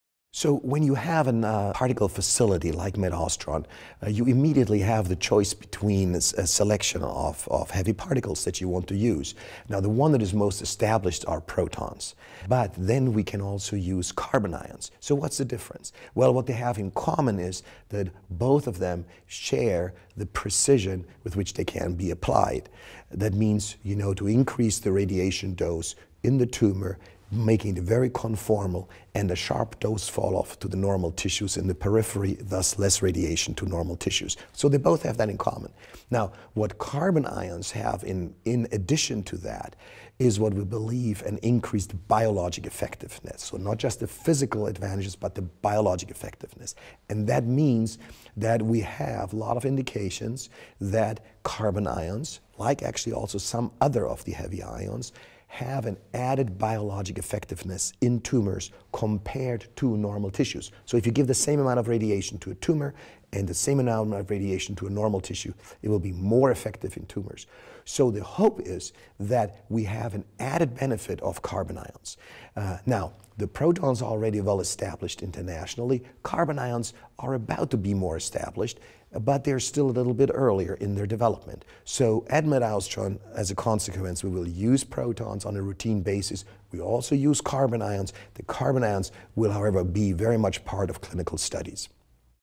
Fast Particles for Fighting Cancer
Prof. Dr. Eugen Hug, Medical Director of MedAustron, explains radiation therapy with charged particles - protons or carbon ions.